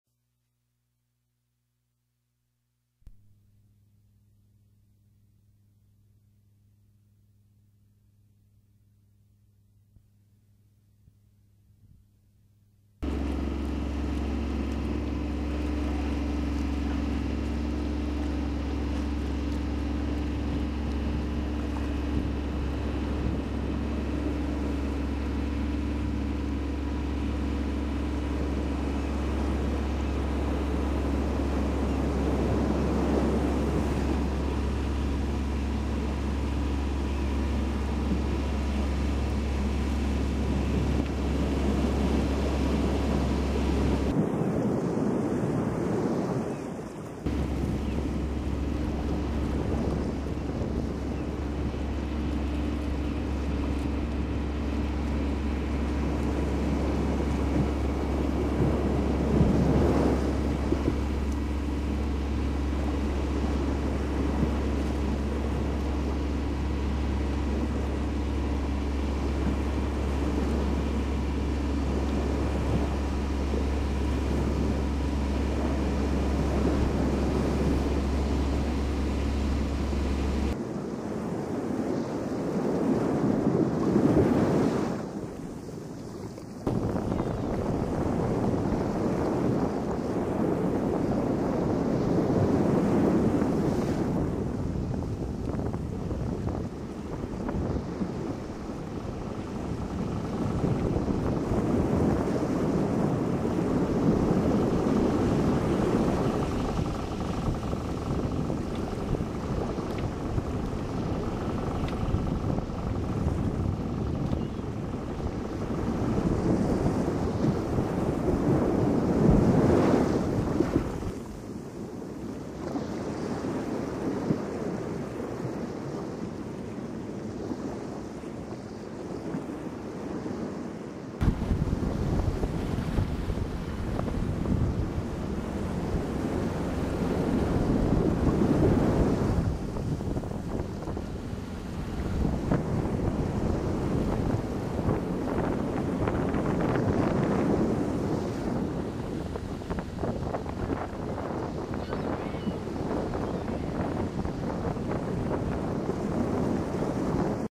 Coromandel Harbour Sailing
Coromandel harbour is 30 NM from Auckland NZ and is enjoyed by yachts and fishing boats. Coromandel Town ferry has regular connections to Hanfords Jetty.